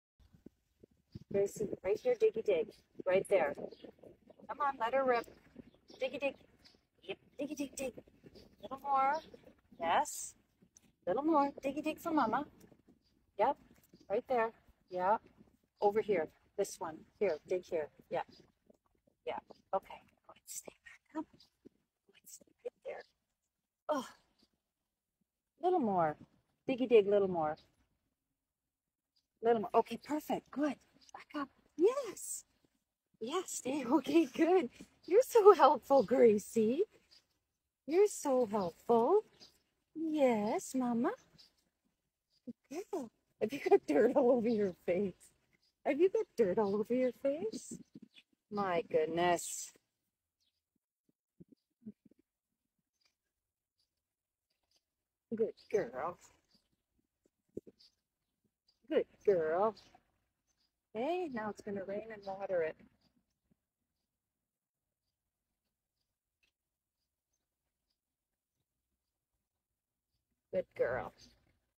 Pup Helps Plant Flowers || ViralHog
Occurred on June 11, 2022 / Mississauga, Ontario, Canada

"18-month-old amazing Graycie helping mama plant flowers."